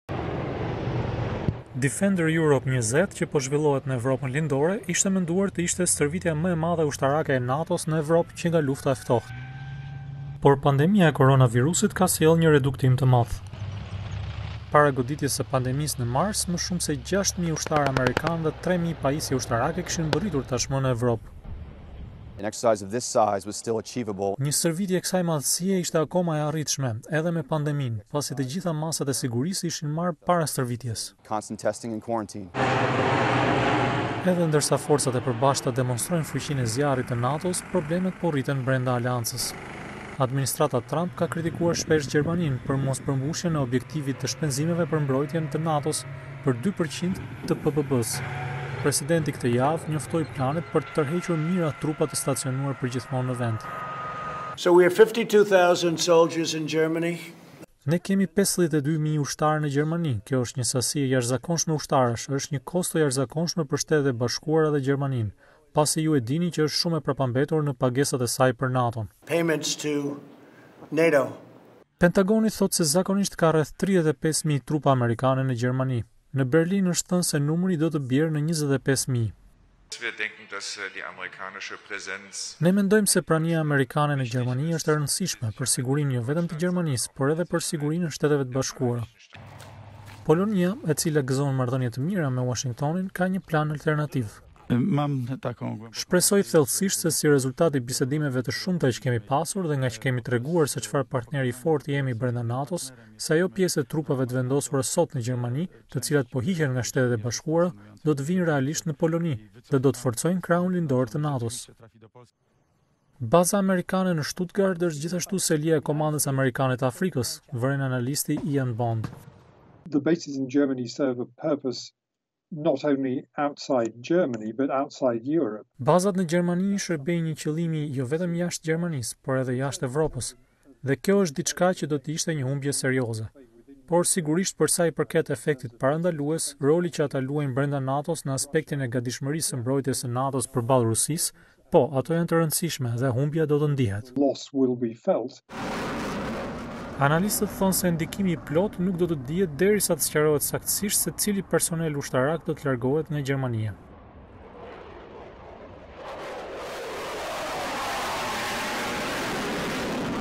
Plani për tërheqjen e forcave amerikane lë në hije stërvitjen e NATO-s
Mijëra trupa amerikane po marrin pjesë në stërvitjet ushtarake të NATO-s në Evropën Lindore. Një pjesë e trajnimit po zhvillohet në Poloni, vetëm 350 kilometra larg enklavës ruse të Kaliningradit, ndërsa stërvitjet detare po zhvillohen gjithashtu në Detin Baltik. Korrespondenti i Zërit të Amerikës, Henry Ridgwell njofton nga Londra se ky demonstrim force është lënë në hije nga njoftimi i Uashingtonit se planifikon të tërheqë mijëra forca ushtarake nga Gjermania